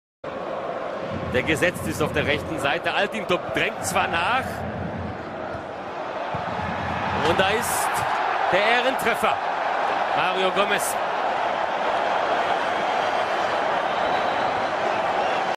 Mario Gómez and Martin Lanig
VFB Stuttgart - Bayern Munchen 1-5 DFB Pokal, Mario Gómez goals and congratulations Martin Lanig